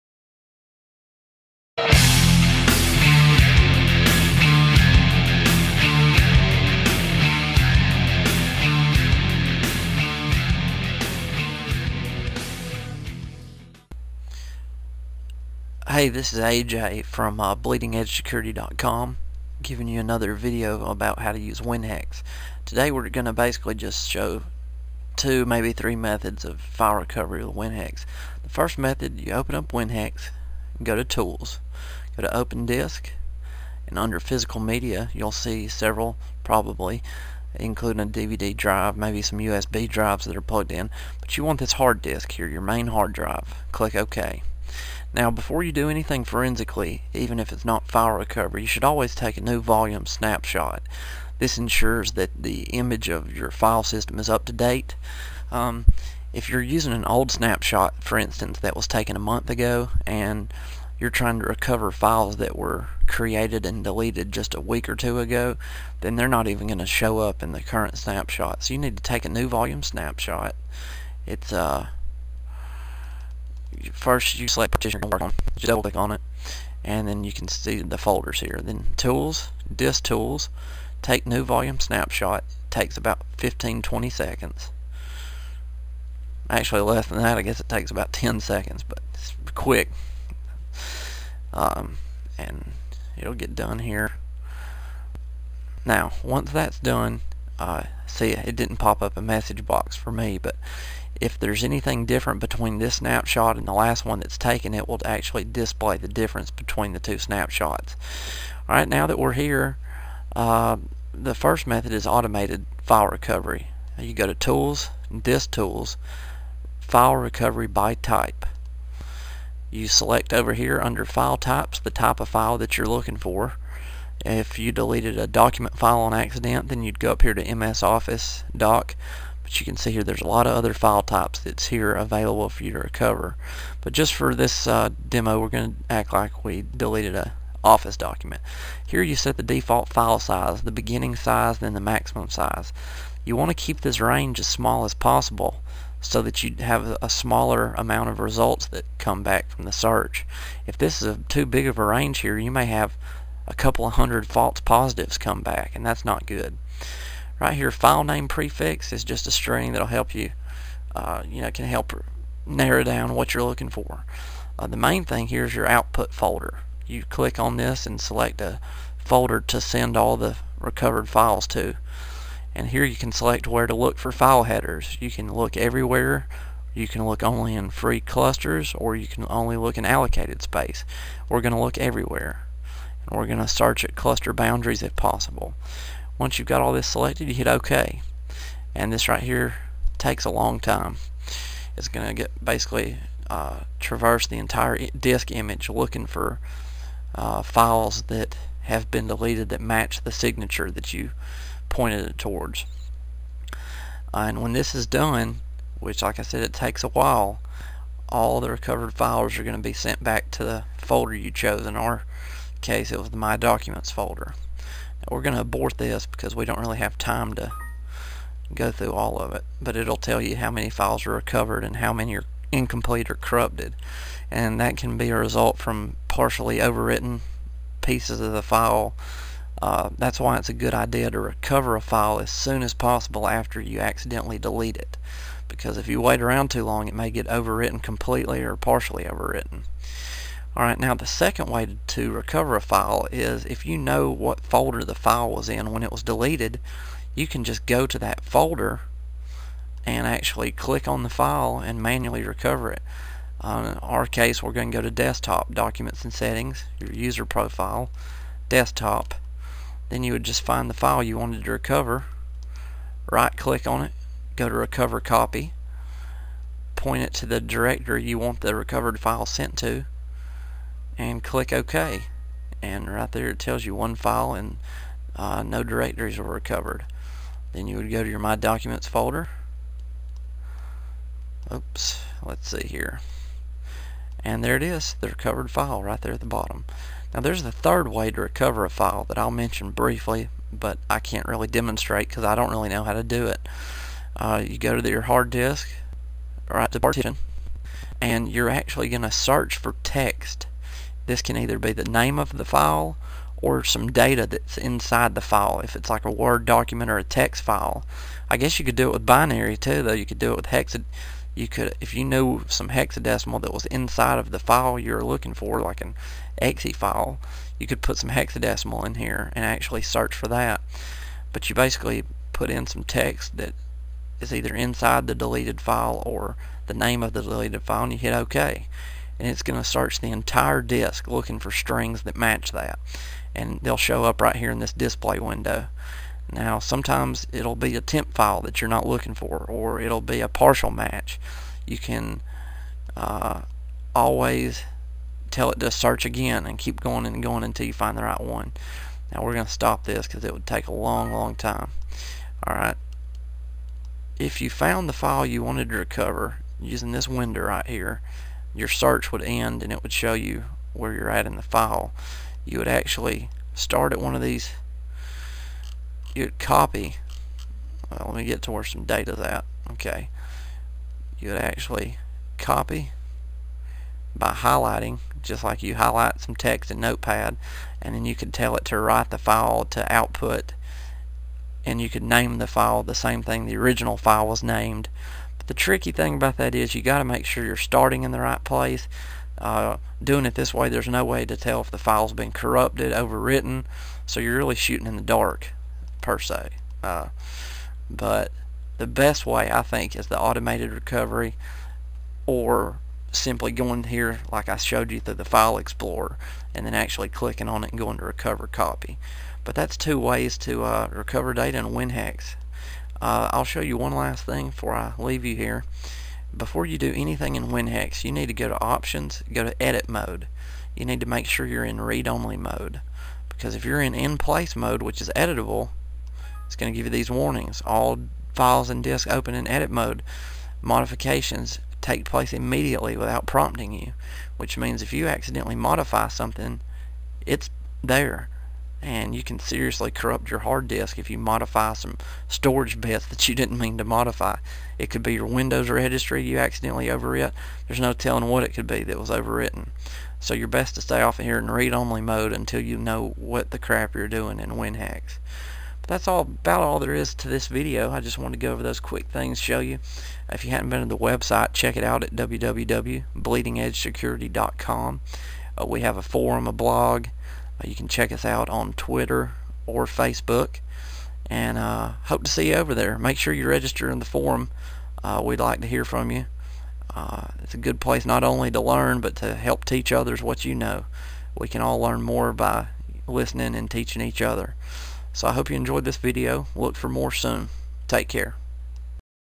Tutorial: Digital Forensics with Winhex Part 2
Learn basics about Digital Forensics.

Blog This video demonstrates how to use WinHex to recover deleted data from the hard disk. Three methods are shown.